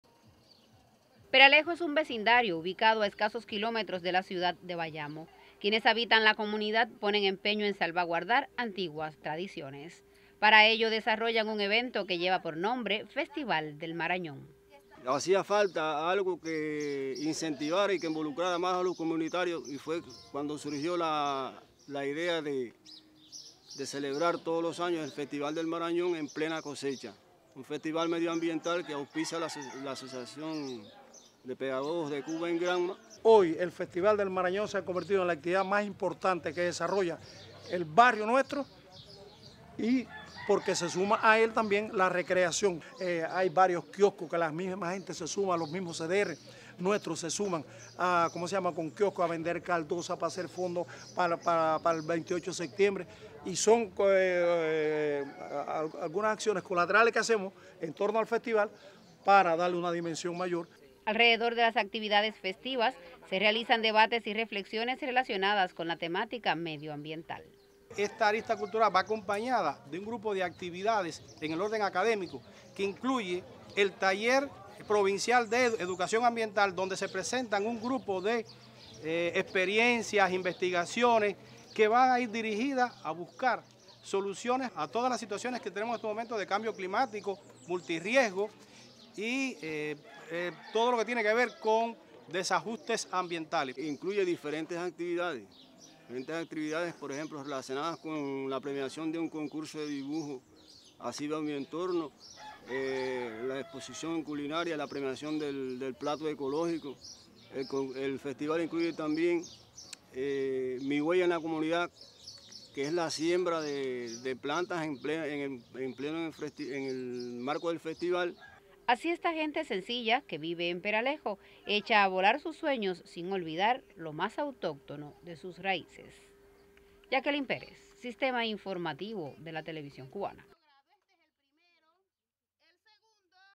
Festival rescata tradición bayamesa en el uso del marañón
Con un alto componente cultural, transcurrió, este fin de semana, la Quinta Edición del Festival del Marañón, en la comunidad de Peralejo, en Bayamo. El evento propició el debate sobre los resultados  en la siembra y propagación de esta fruta, al tiempo que se expusieron las variedades de dulces y productos elaborados a base del marañón, como vinos, rones y jaleas.